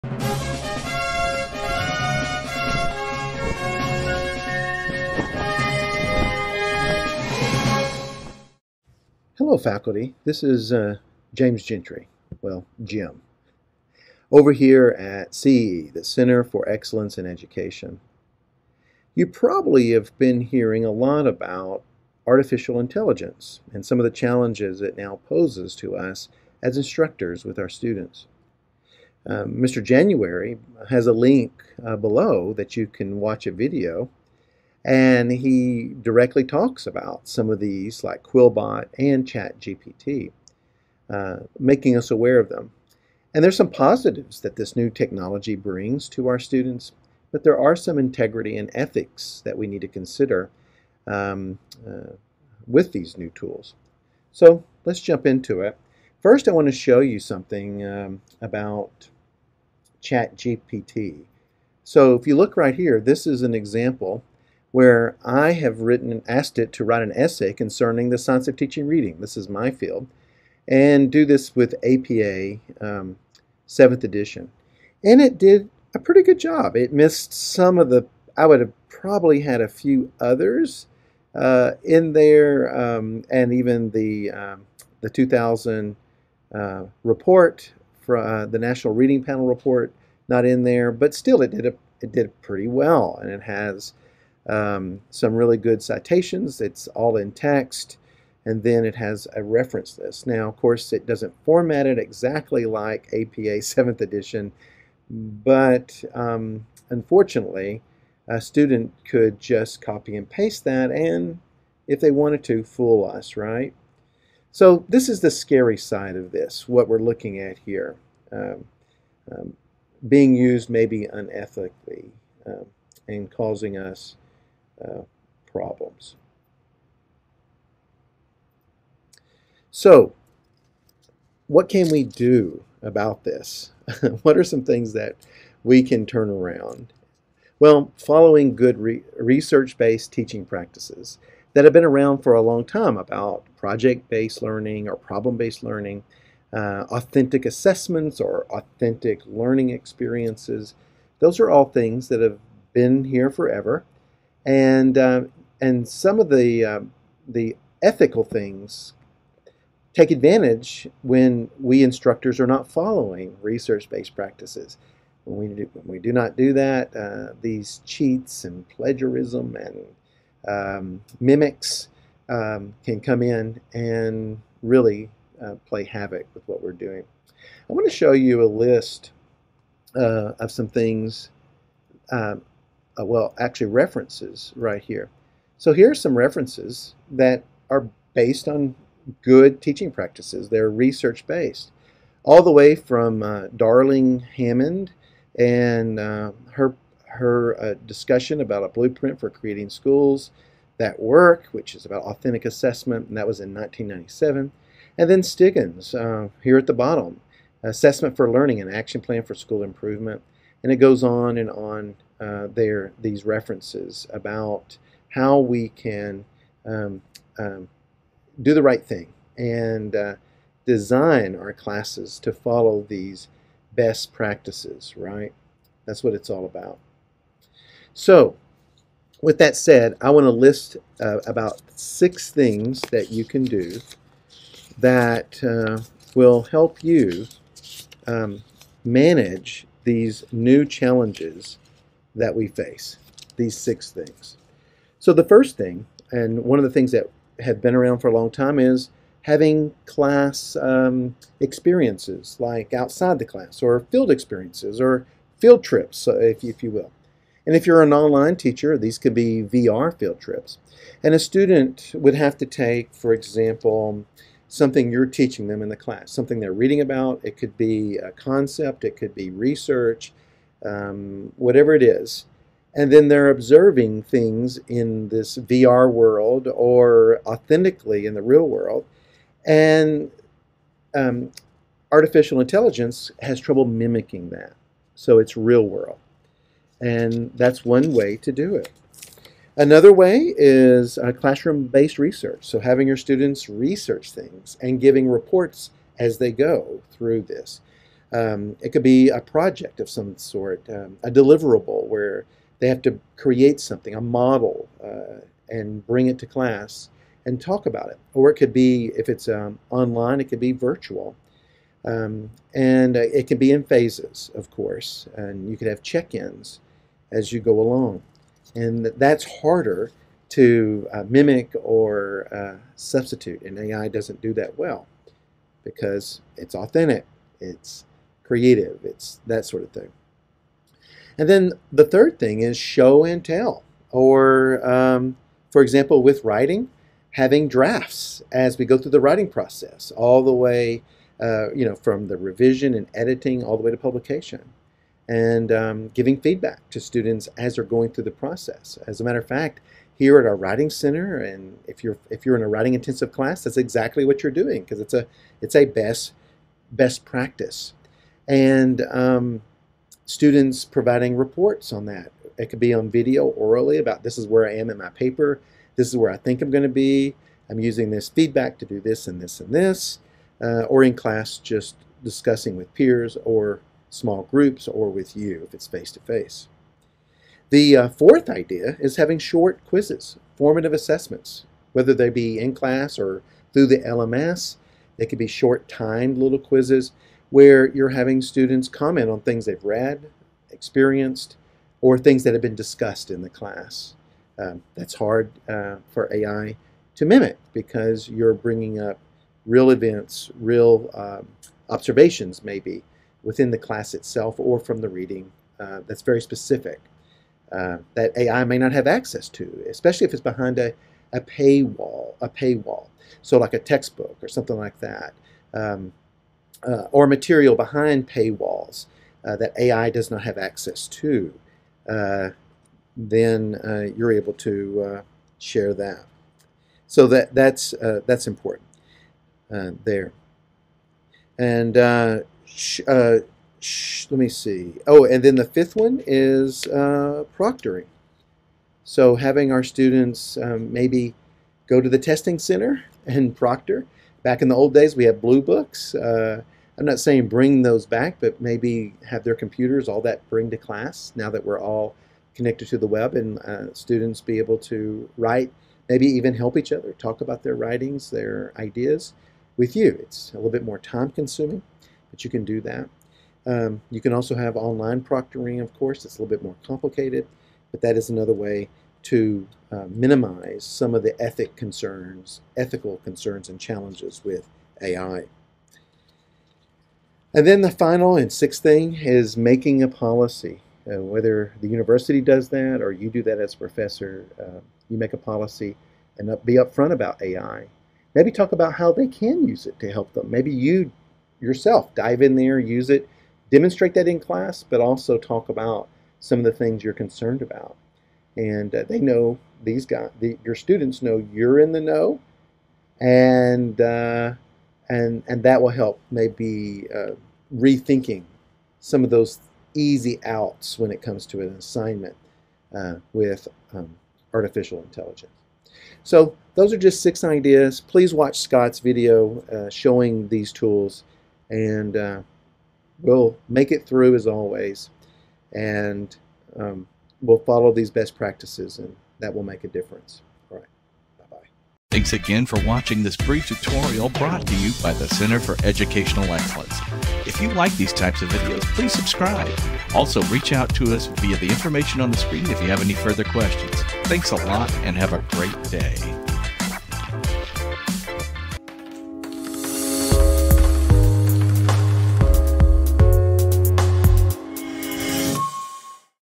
Video Discussion - Ethics using Ai Tools
In this video Dr. Jim Gentry discusses six best practices in the use of Ai tools.